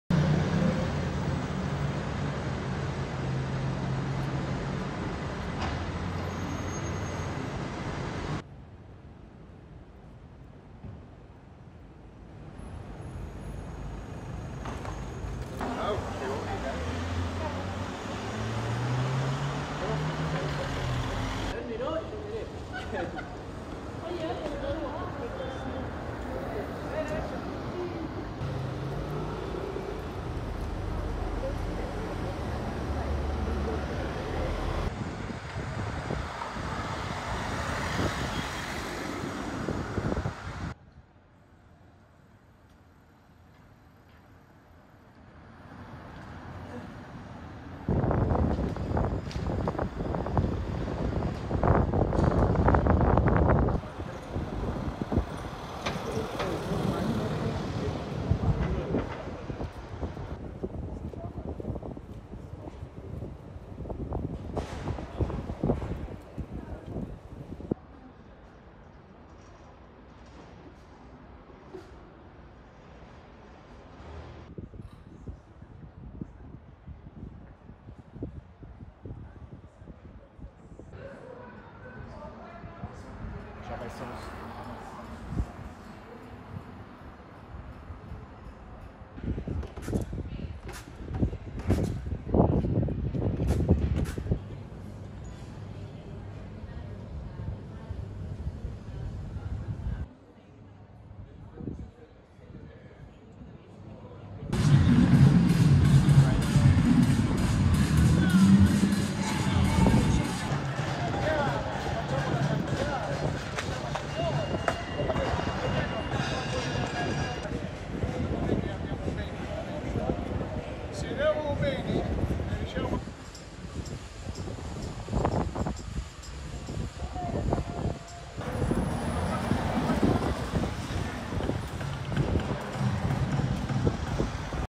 Londra'daki boş cadde ve sokaklarından detay görüntüler
Hükümetin koronavirüs salgınıyla mücadele kapsamında halka sosyalleşmeme tavsiyesinde bulunmasının ardından Londra'da cafe, bar, restoran ve merkezi caddelere devam edenlerin sayısı azaldı.